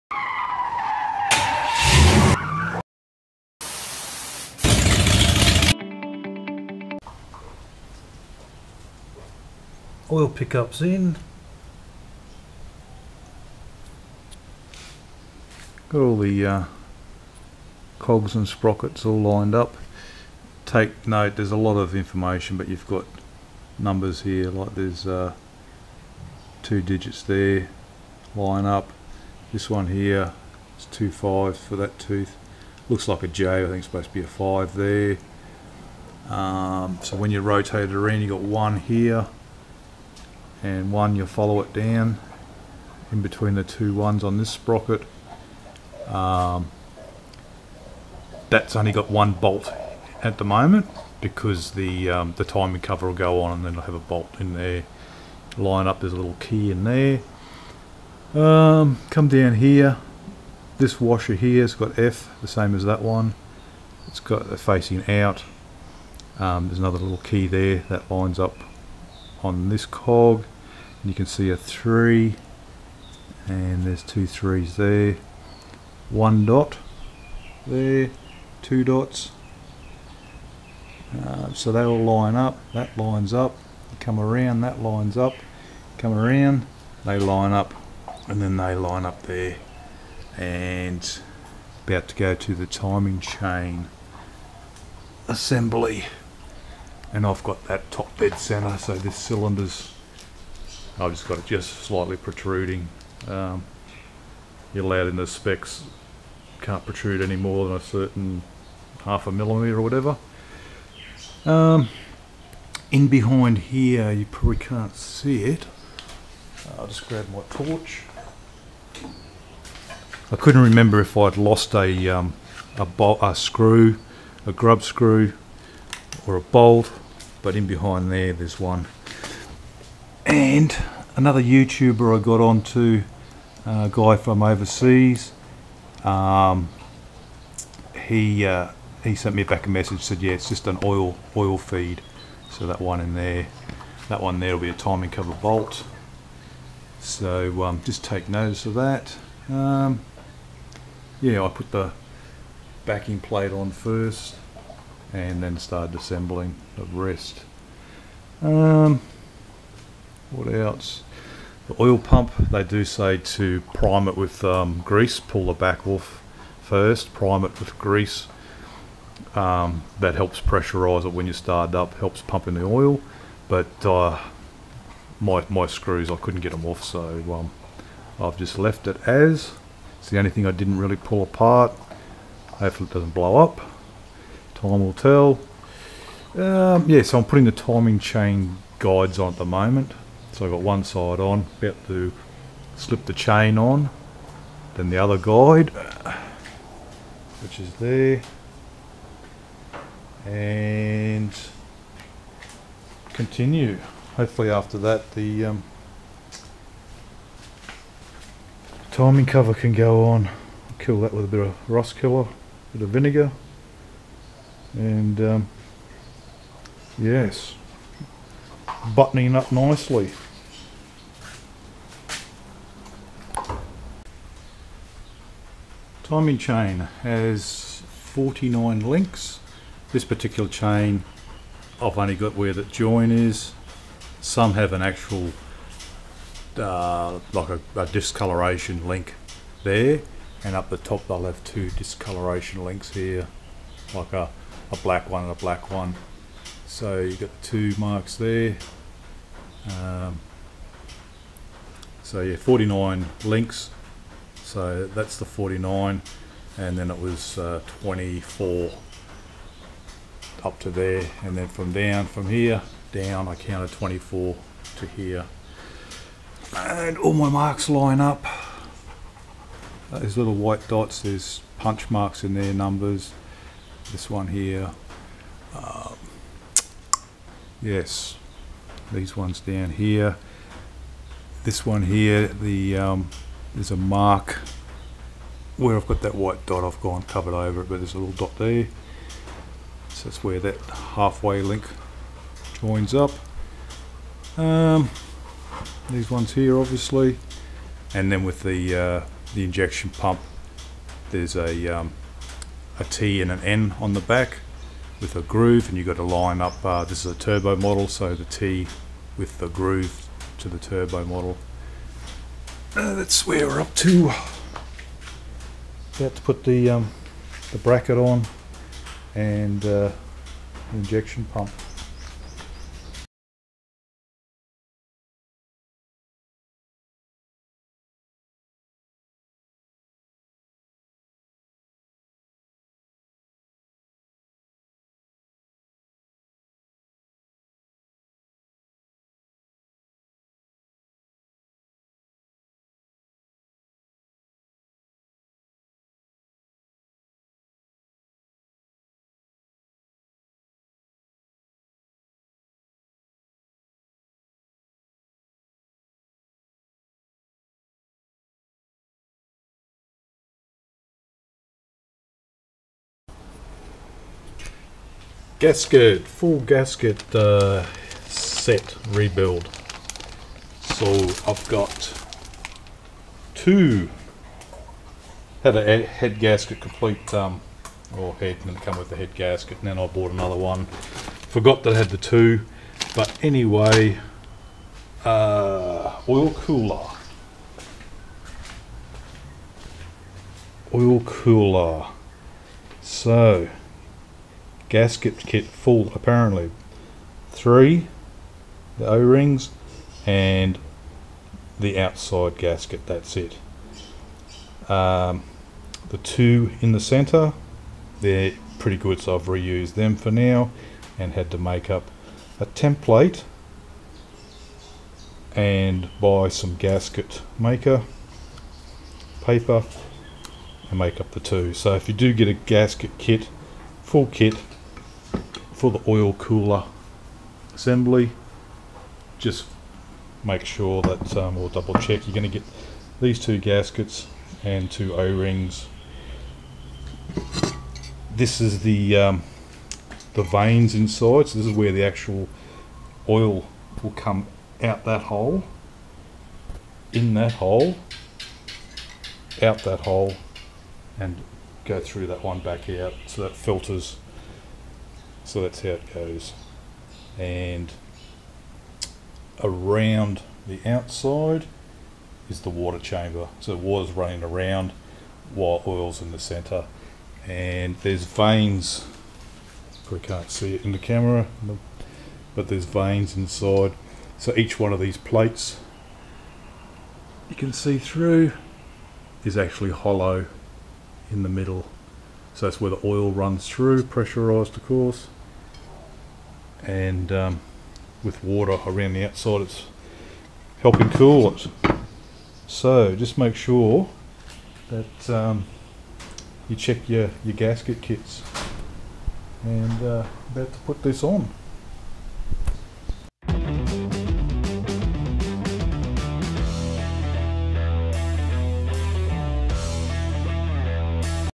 MITSUBISHI NJ Pajero 94 PART 3 ENGINE REBUILD 2.8LT 4M40T
MITSUBISHI NJ  PAJERO  4M40T "ENGINE  REBUILD"
CRANK SHAFT AND TIMIMNG MARKS
NEW TIMING CHAIN AND GUIDES. NEW OIL COOLING HOUSING
GASKETS.